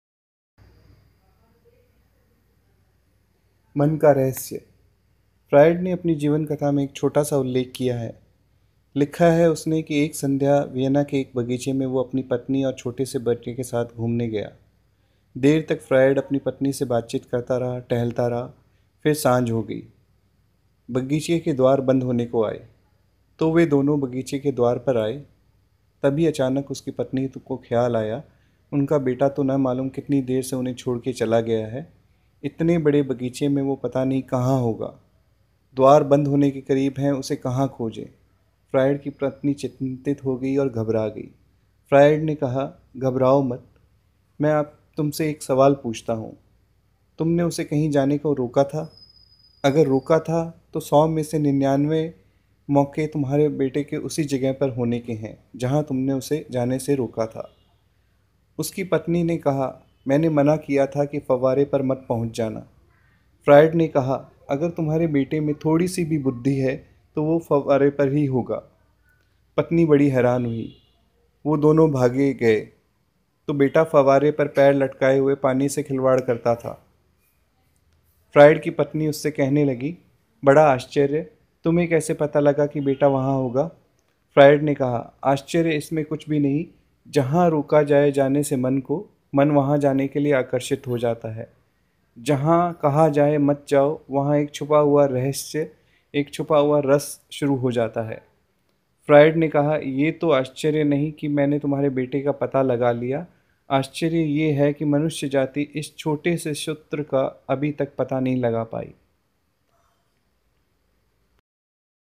Biggest secret of Mind
the small story which tells a biggest truth about our  mind that what ever restricted it attracts to it